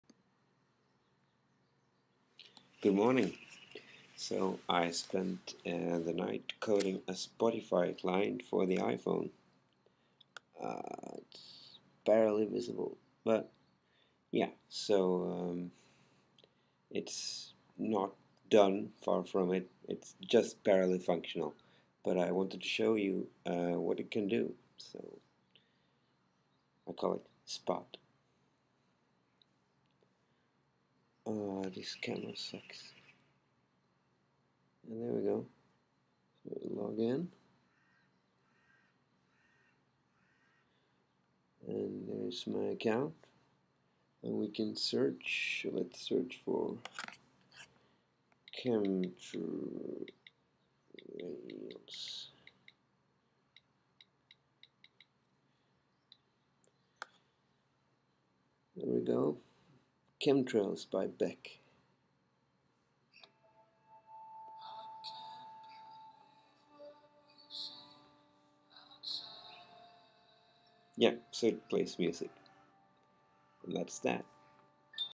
The Birth of Spot, the iPhone Spotify Client
I spent the night hacking some Cocoa, trying to get despotify (despotify.se) working on the iPhone. A few hours on the UI and *quite* a few hours on the AudioQueue driver, and it's working!